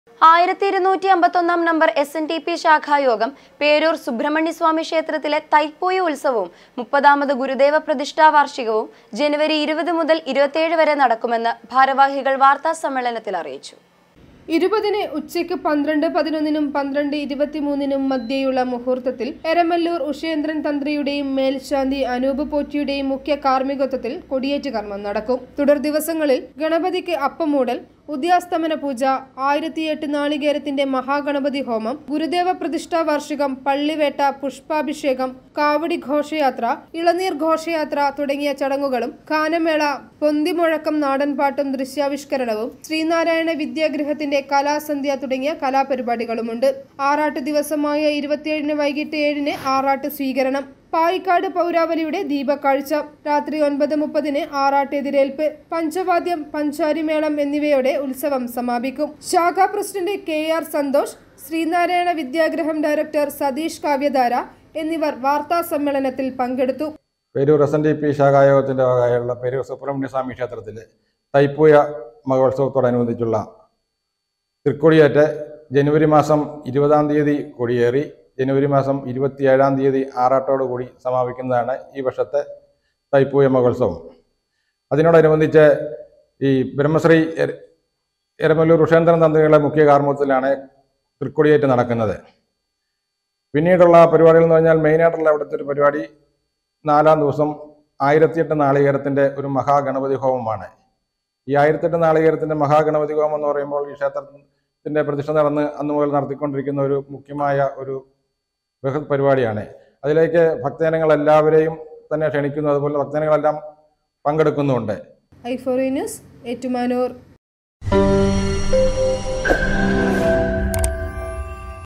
പേരൂര്‍ സുബ്രഹ്മണ്യസ്വാമി ക്ഷേത്രത്തിലെ തൈപ്പൂയ ഉത്സവും 30-ാമത് ഗുരുദേവ പ്രതിഷ്ഠാ വാര്‍ഷികവും
1251-ാം നമ്പര്‍ എസ്.എന്‍.ഡി.പി. ശാഖായോഗം പേരൂര്‍ സുബ്രഹ്മണ്യസ്വാമി ക്ഷേത്രത്തിലെ  തൈപ്പൂയ ഉത്സവും
 30-ാമത് ഗുരുദേവ പ്രതിഷ്ഠാ വാര്‍ഷികവും
ജനുവരി 20 മുതല്‍ 27 വരെ നടക്കുമെന്ന്
ഭാരവാഹികള്‍ വാര്‍ത്ത സമ്മേളനത്തില്‍ അറിയിച്ചു